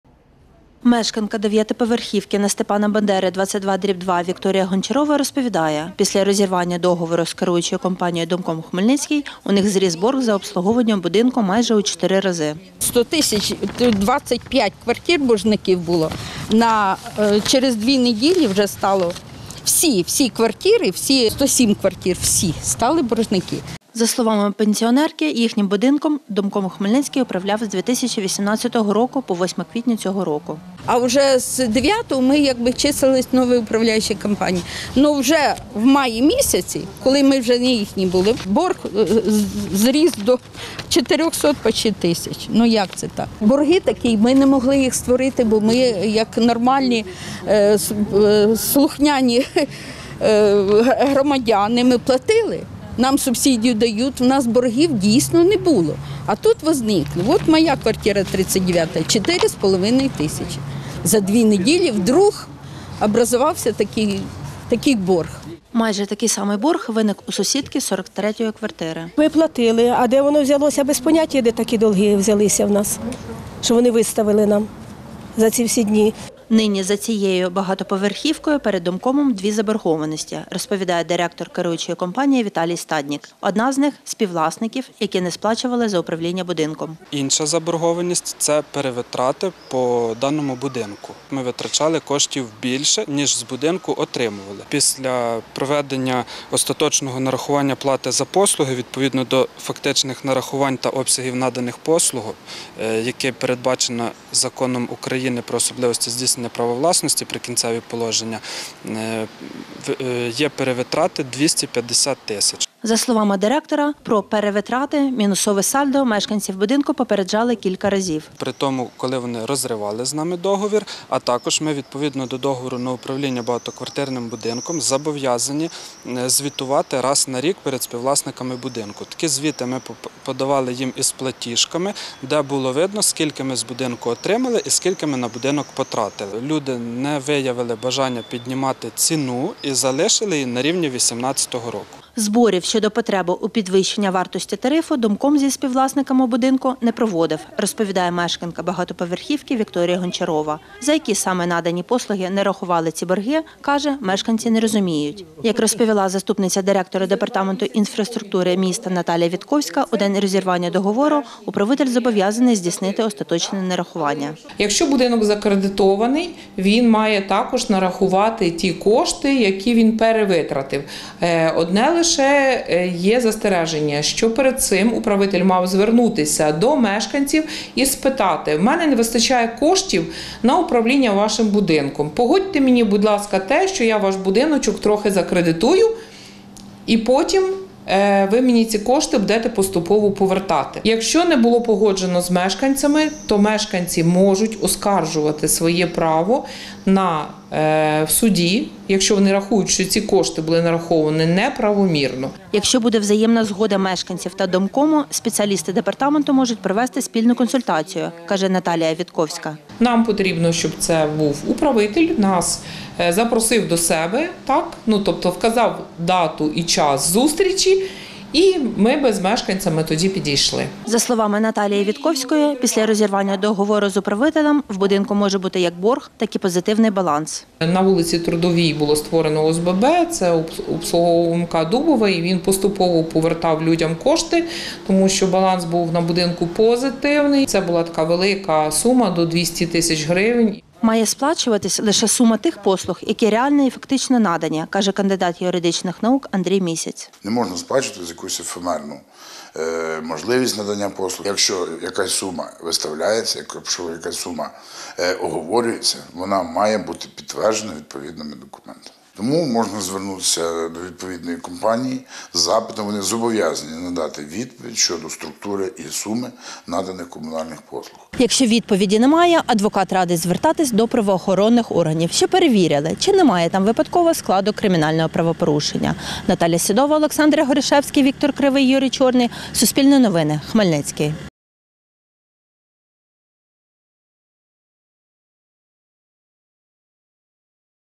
Борг за обслуговування будинку зріс учетверо після розірвання договору з управляючою компанією
Після розірвання договору з керуючою компанією «ДомКом Хмельницький», у мешканців однієї з багатоповерхівок обласного центру зріс борг зі 100 тисяч до 385 тисяч гривень. Про це Суспільному розповіла одна з мешканок цієї дев’ятиповерхівки Вікторія Гончарова. За словами пенсіонерки, вони не розуміють причин такого стрімкого зростання заборгованості. Детальніше – у сюжеті «Суспільного».